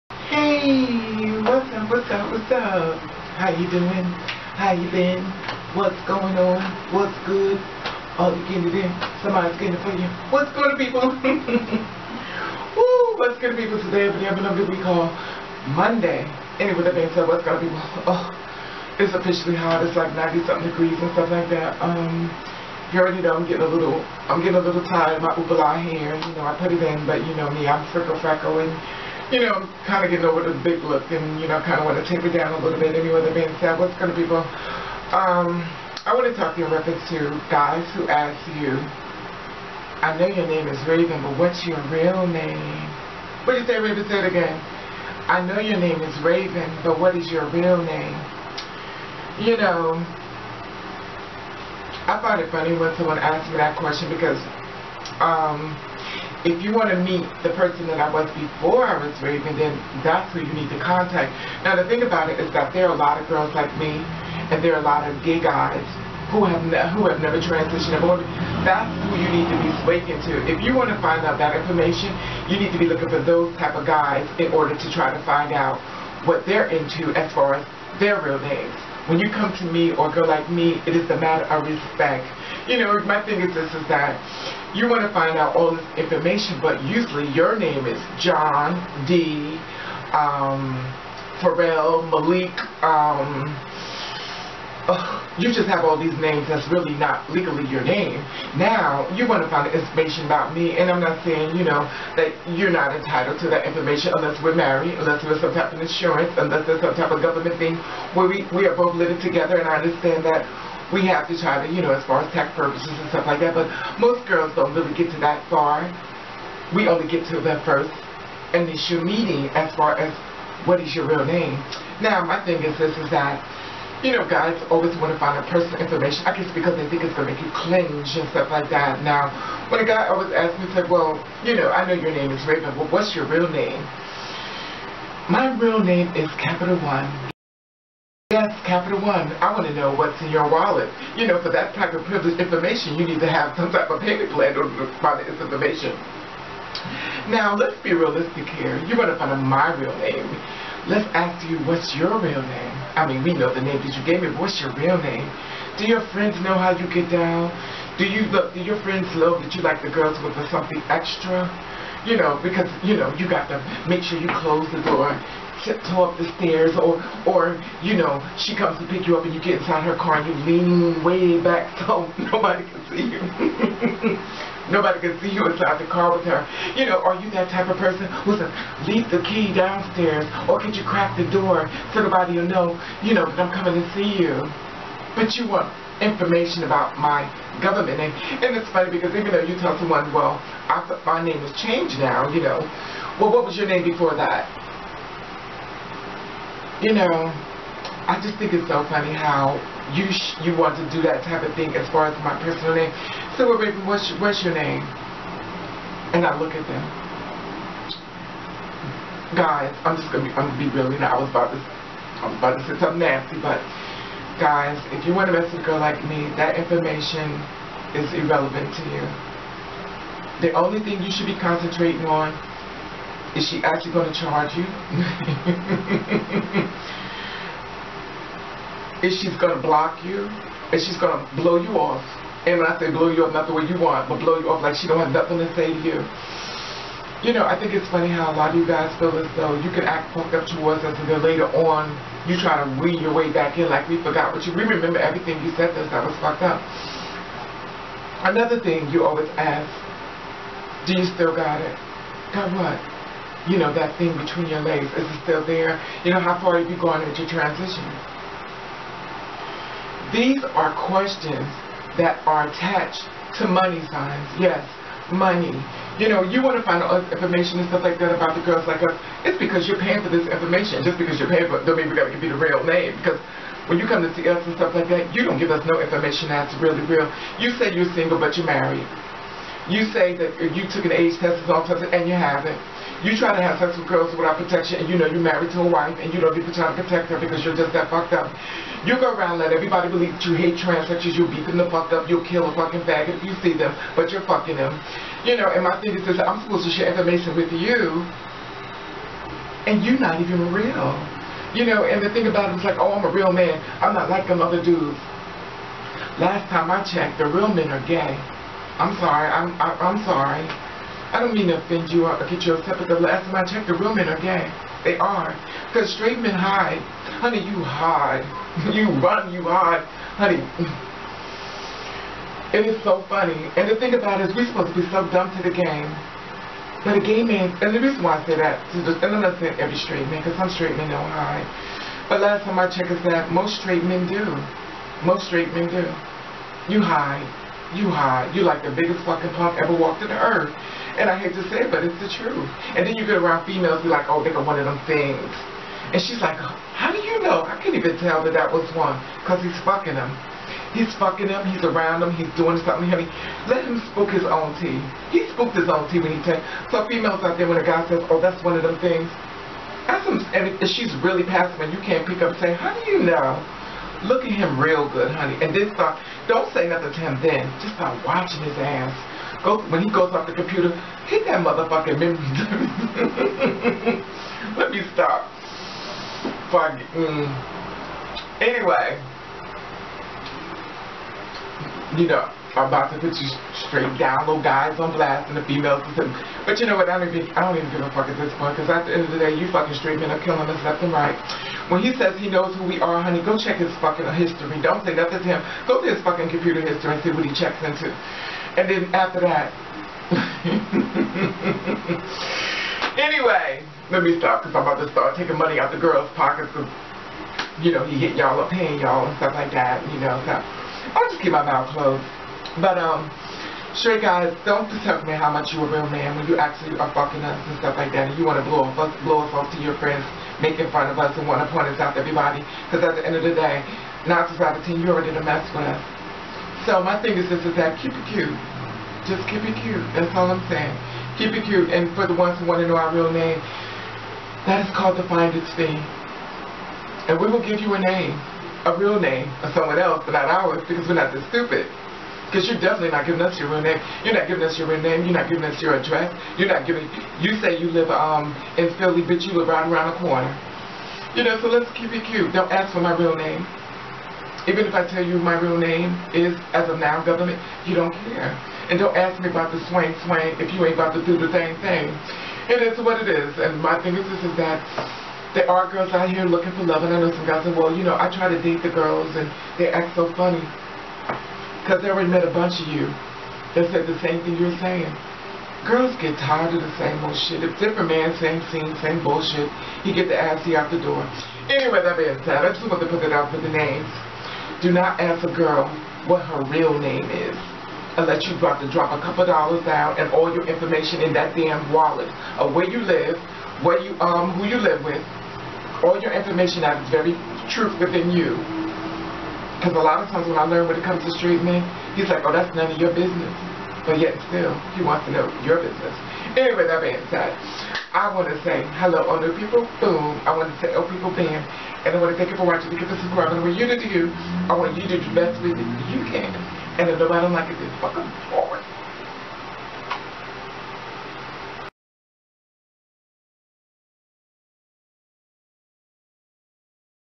Never ask a Transgender what is your real name unless
This video is about how men like to disrespect trans women when they asked her, her name and she tell him but he wants the male name and not the name she lives by well let's watch